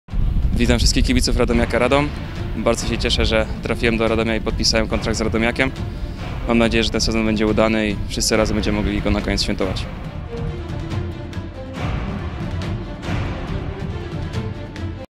Jakub Wawszczyk zawodnikiem Radomiaka!
Jakub Wawszczyk został nowym zawodnikiem Radomiaka. 20-letni lewy obrońca został wypożyczony z Arki Gdynia do końca sezonu 2018/2019.

Jakub Wawszczyk jesienią minionego sezonu reprezentował pierwszoligową Olimpię Grudziądz, a wiosną przeniósł się do KP Starogard Gdański. Wcześniej notował też występy w KS Chwaszczyno i rezerwach Arki Gdynia.

Witamy w Radomiaku!